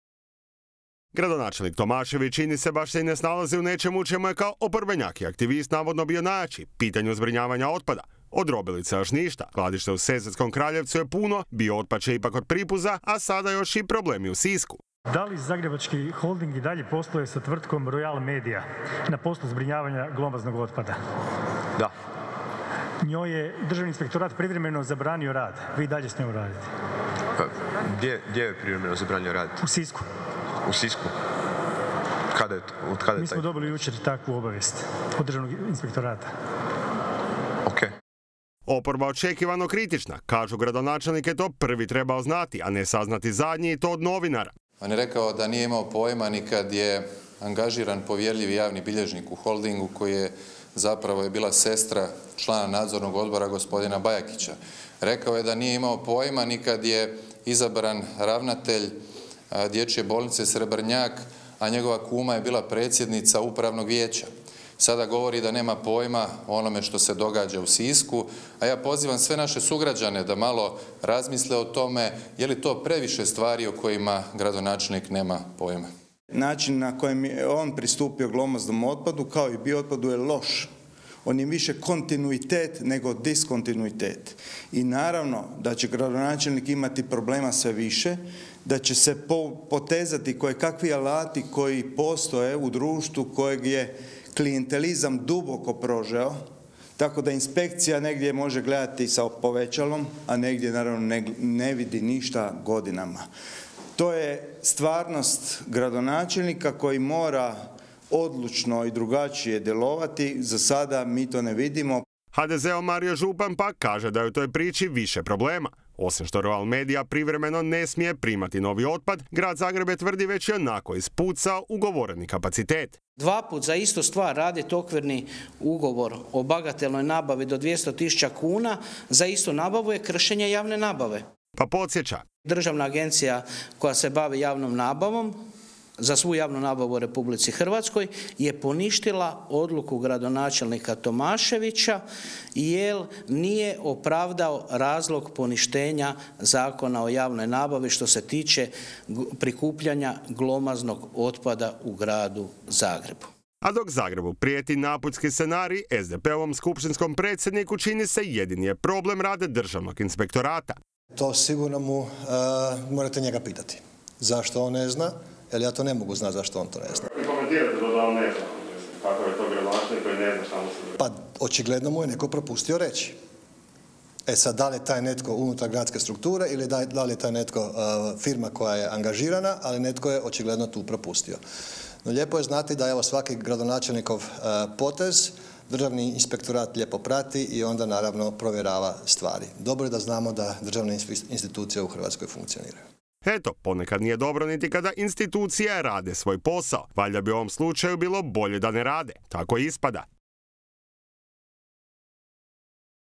Gradonačelnik Tomašević u otpadu do vrata!
Gradonačelnik Tomašević, čini se, baš se i ne snalazi u nečemu u čemu je, kao oporbenjak i aktivist, navodno, bio najjači - pitanju zbrinjavanja otpada. Od drobilica još ništa, skladište u Sesvetskom Kraljevcu je puno, biootpad će ipak kod Pripuza - a sada i problemi u Sisku.